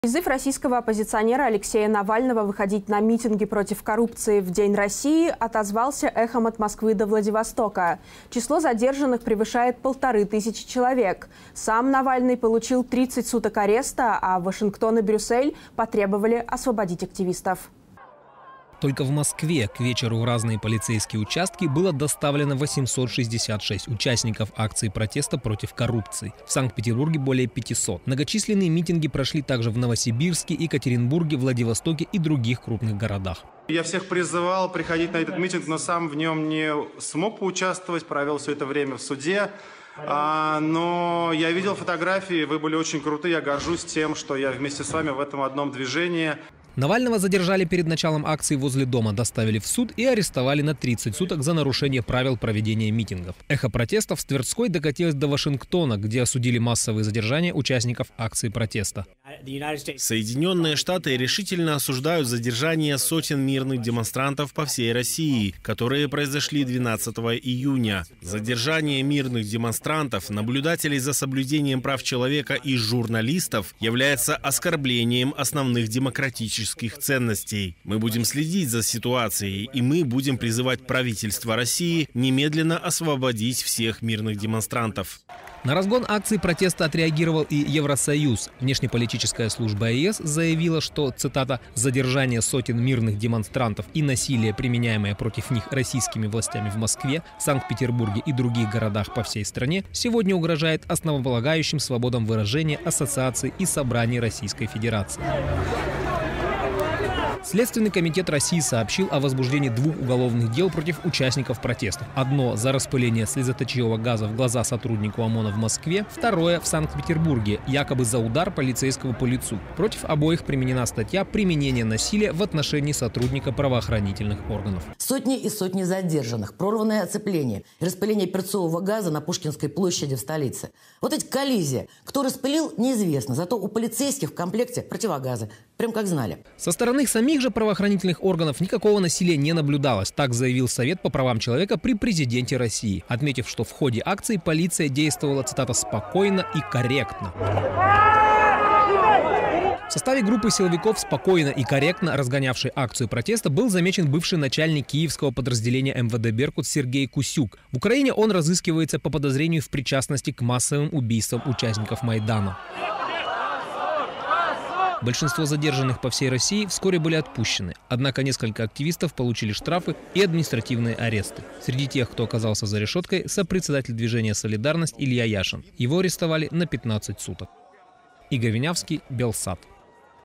США и ЕС осудили избиение участников антикоррупционной акции в России / Вот так
Призыв российского оппозиционера Алексея Навального выходить на митинги против коррупции в День России громким раскатом прошел от Москвы до Владивостока. Число задержаных превышает тысячу человек. Cам Навальный получил 30 суток ареста, а Вашингтон и Брюссель потребовали освободить активистов.